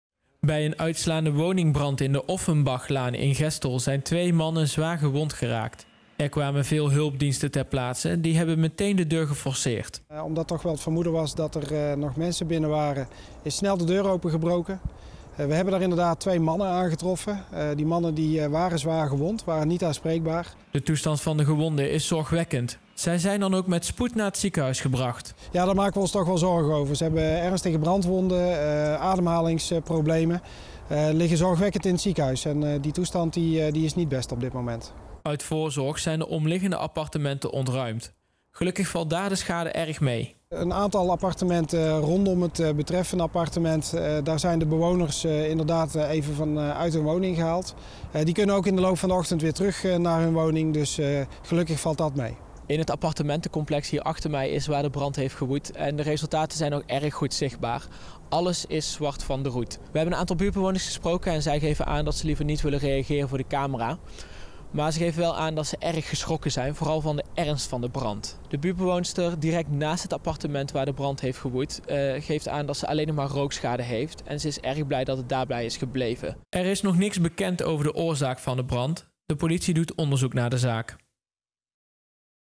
Twee zwaargewonden bij brand Offenbachlaan
Bij een fikse brand aan de Offenbachlaan zijn twee mannen zwaargewond geraakt. Ze zijn kritieke toestand naar het ziekenhuis gebracht.